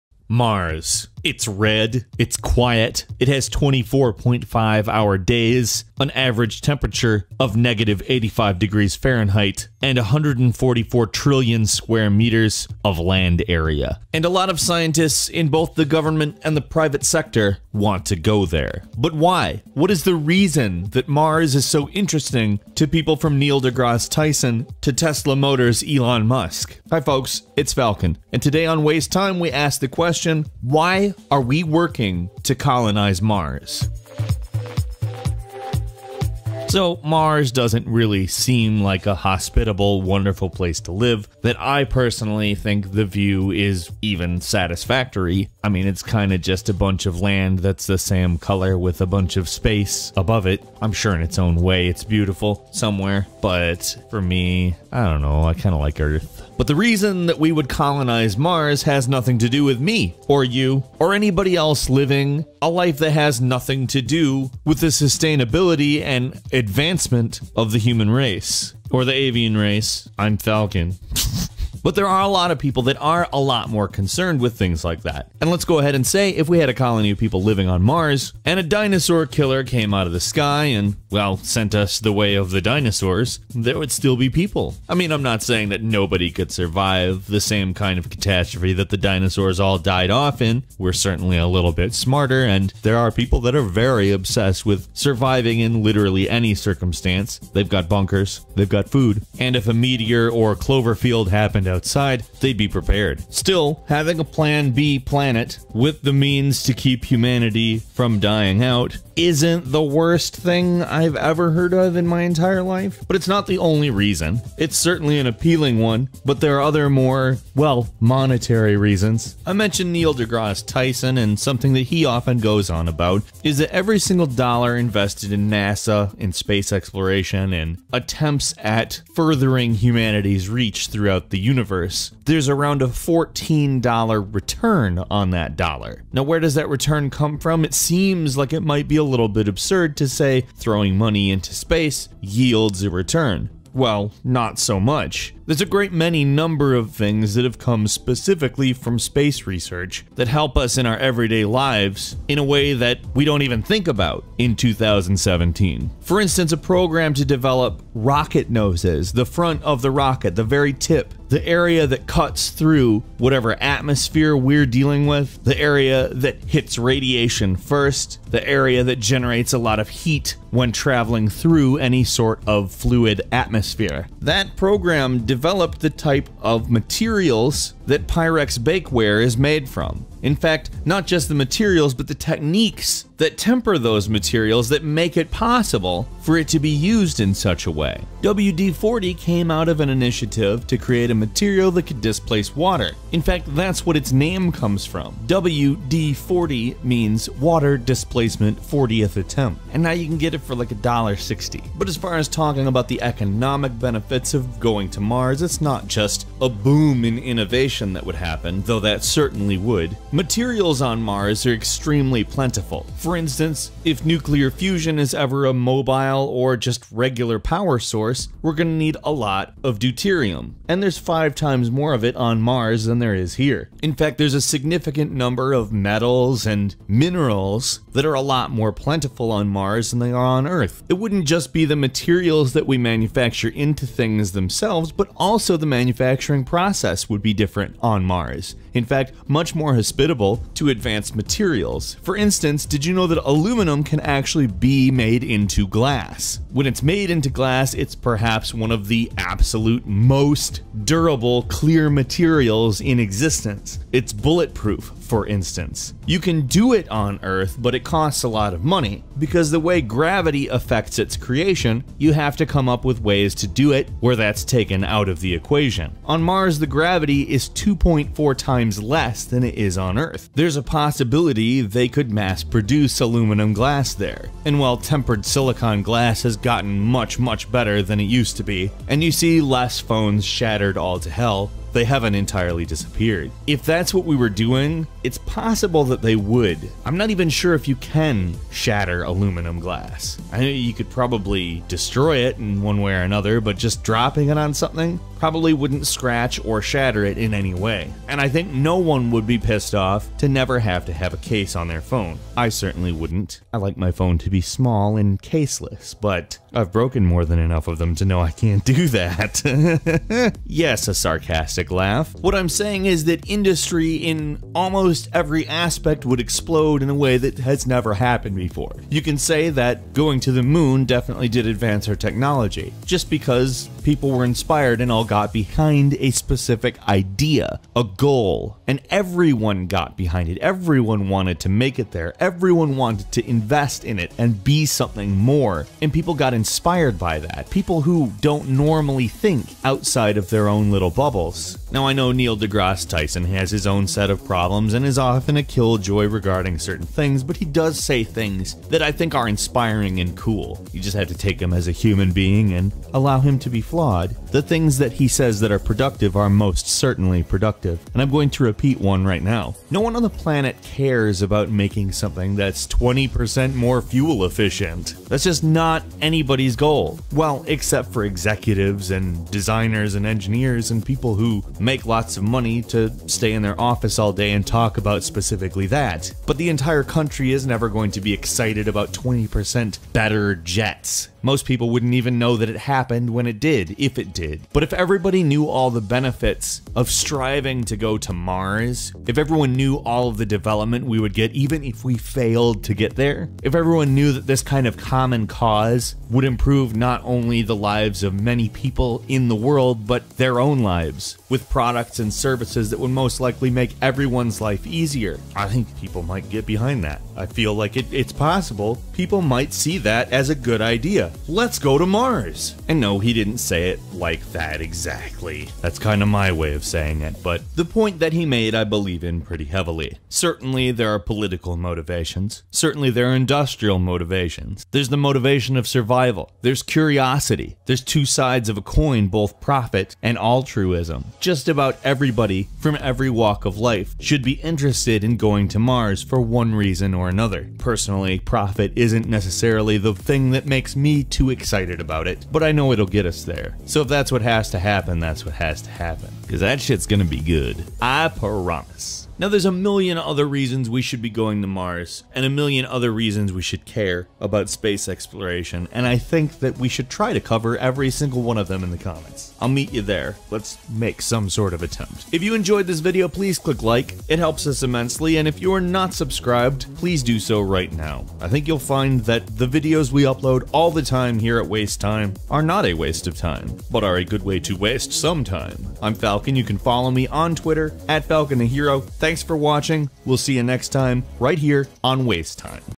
Why Are We Working To Colonize Mars?
Besides heading to space and pursuing our destiny in the stars, there's good reasons to colonize Mars. Here's why we're doing it.